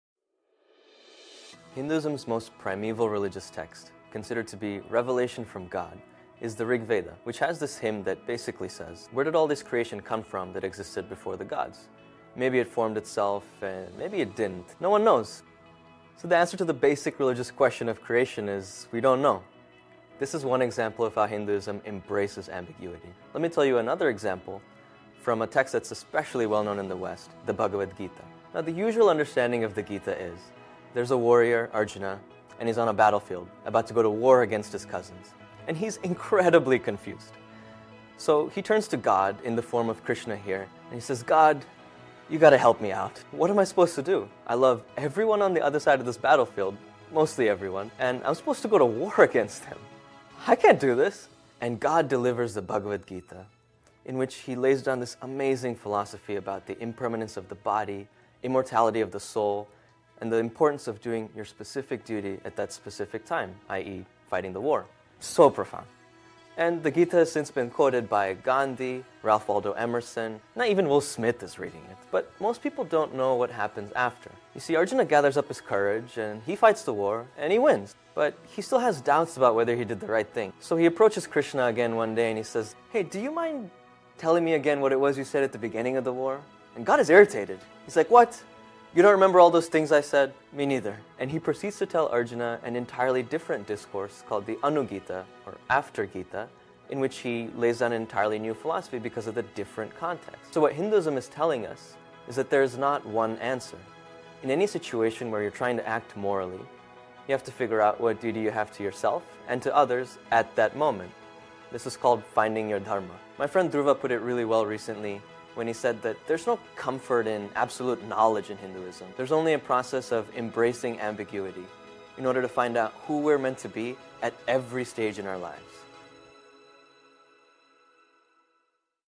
How Hinduism Embraces Ambiguity
What if religion taught us to embrace ambiguity instead of giving us the answers? Harvard Divinity School student Abhijith Ravinutala, MTS ’19, gives the 2-minute story on how Hinduism helps us find our way in a world of uncertainty.

Featuring: Abhijith (Abhi) Ravinutala, MTS ’19
Producer: Jade Rei Sylvan, MDiv ’20

Learn more about the ways that HDS makes a world of difference